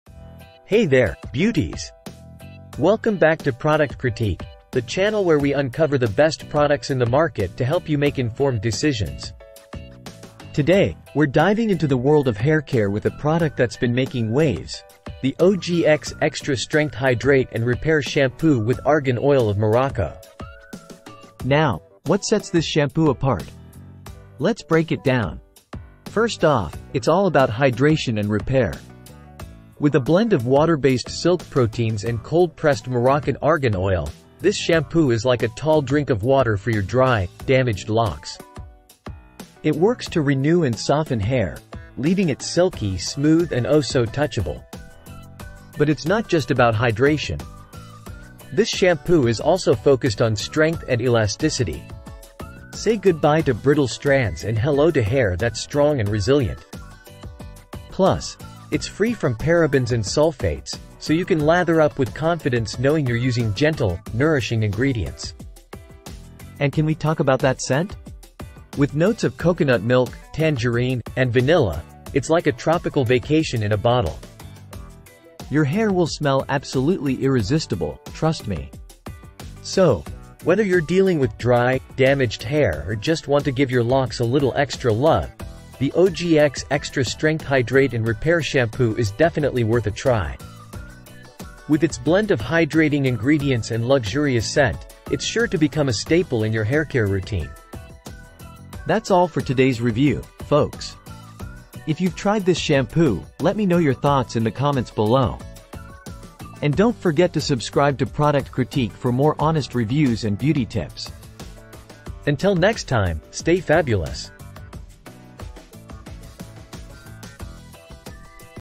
🌿 OGX Extra Strength Hydrate & Repair + Argan Oil | Best Argan Oil of Morocco Dry Shampoo 🧴
Amazon Product Name : OGX Extra Strength Hydrate & Repair + Argan Oil of Morocco Shampoo

Revitalize your hair between washes with the Best Argan Oil of Morocco Dry Shampoo! This innovative formula absorbs excess oil, refreshes your scalp, and adds volume and texture to your locks, leaving them looking and feeling fresh and rejuvenated. Infused with the nourishing benefits of argan oil, the Best Argan Oil of Morocco Dry Shampoo not only cleanses but also conditions your hair, promoting shine and manageability. Say goodbye to greasy hair days and hello to beautiful, revitalized locks with the Best Argan Oil of Morocco Dry Shampoo!